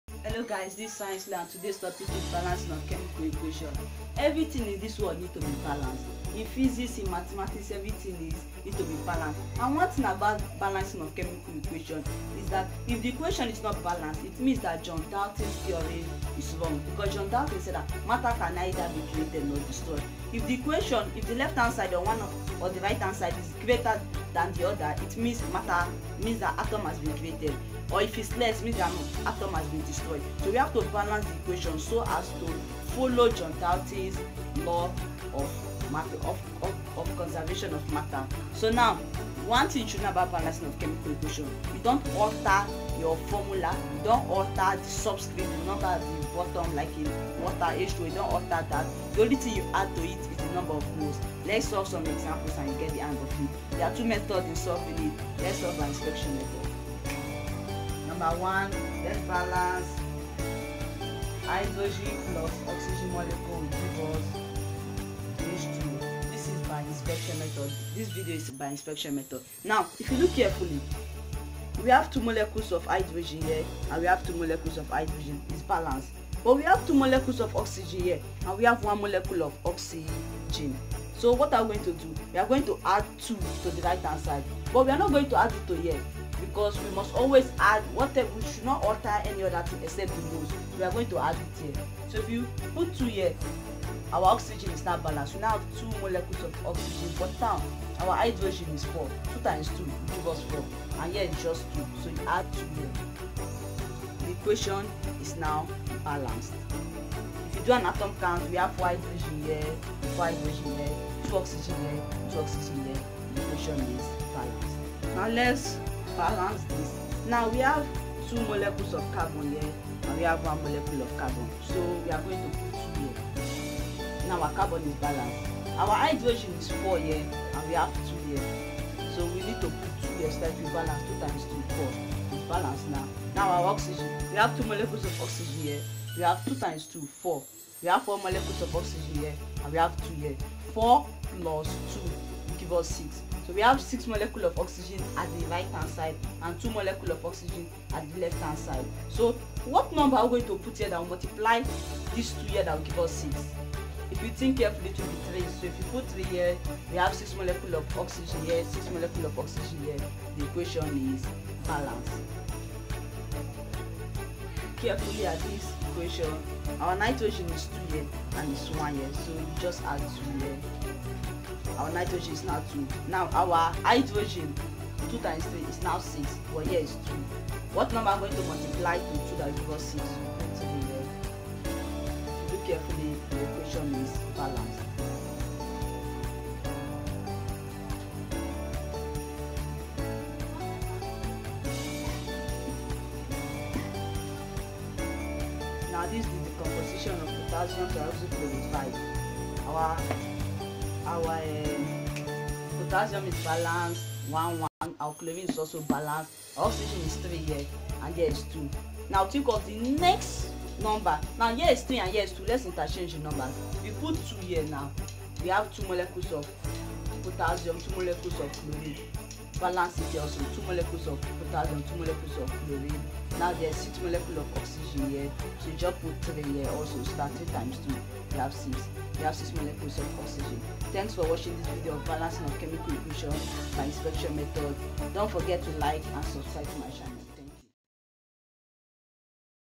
How to balance chemical equations by "inspection method"
This video will help you to balance equations by Inspection.